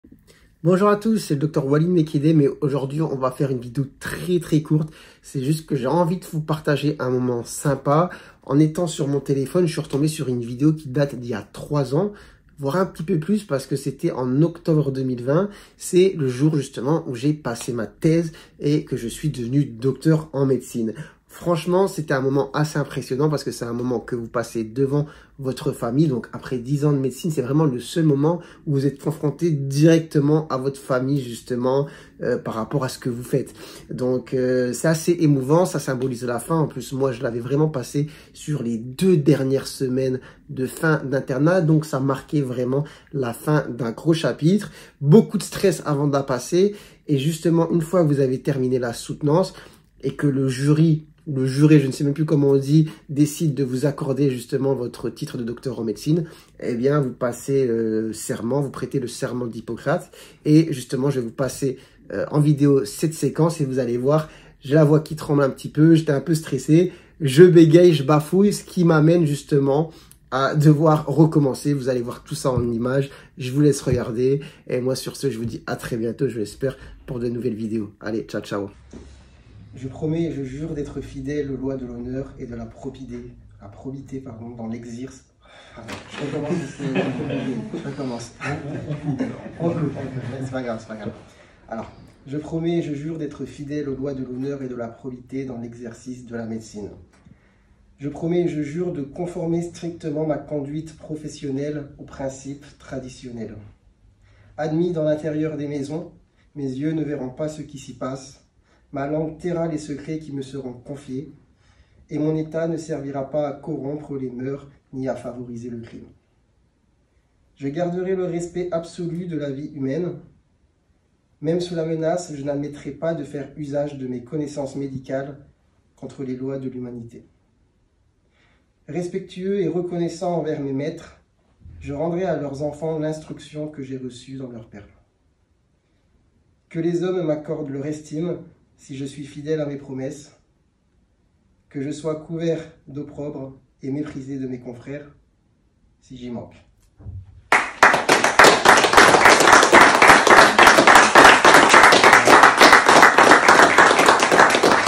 Le jour où je suis devenu DOCTEUR en médecine !
Je vous partage ce souvenir que j'ai retrouvé dans la galerie de mon téléphone !